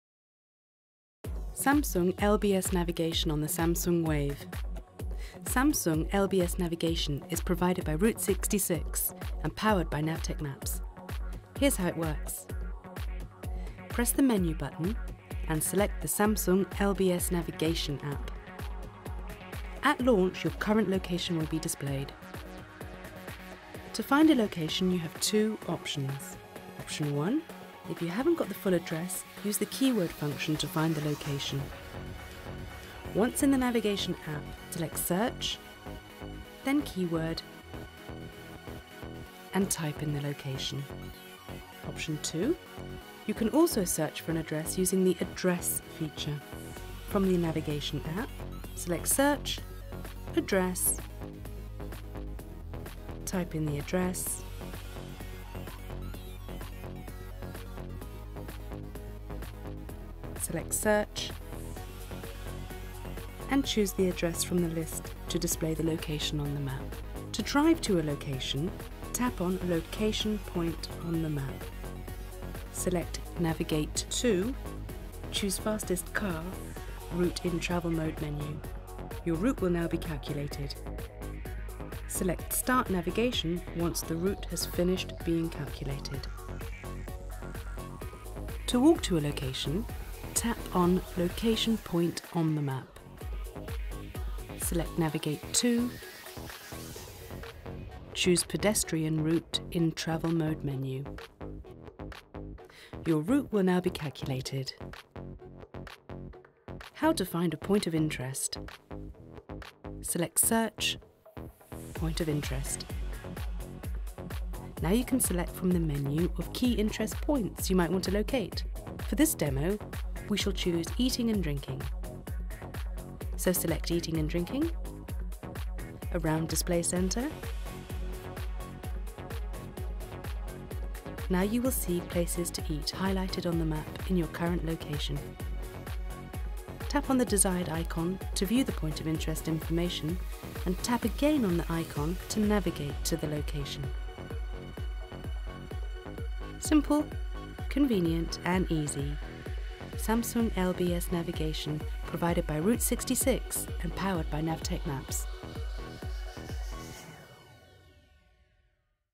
NAVTEQ 2011: Discover How to Use GPS Navigation on the Samsung Wave
A demo video that guides you step by step on how to use navigation on the Samsung Wave.